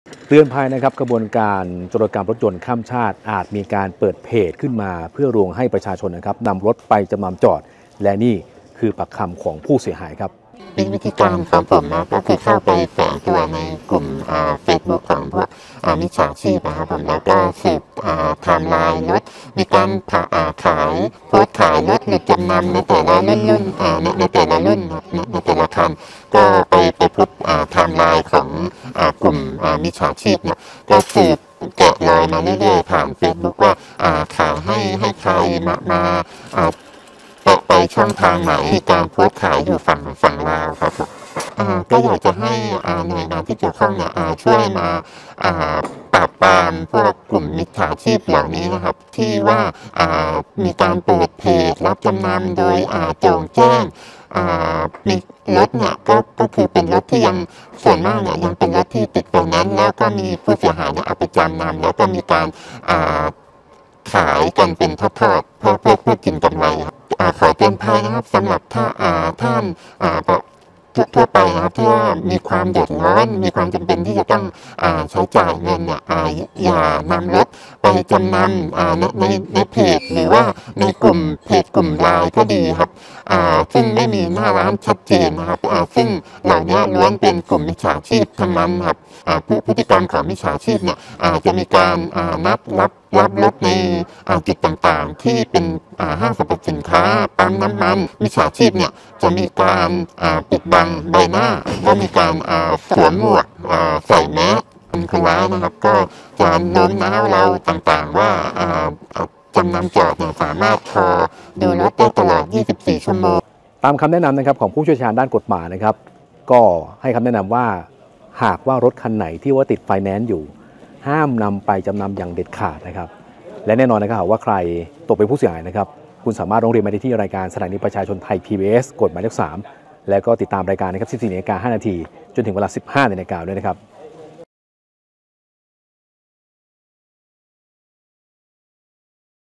ร้องถูกหลอกจำนำรถออนไลน์ ตามสืบเอง พบเบาะแสถูกขายต่อไปถึงลาว | Thai PBS News
ผู้เสียหายถูกผู้เปิดเพจลวงจำนำรถยนต์ผ่านทางออนไลน์ เเละเชิดรถหนี เมื่อติดตามสืบสวนรถยนต์ของตนเอง พบว่าถูกขายทางออนไลน์หลายทอด พบพิกัดสุดท้ายอยู่ริมชายเเดนภาคอีสาน ล่าสุด พบเบาะเเสถูกส่งขายต่อให้นายทุนที่ประเทศลาว ร้องหน่วยงานที่เกี่ยวข้องตามรถกลับคืน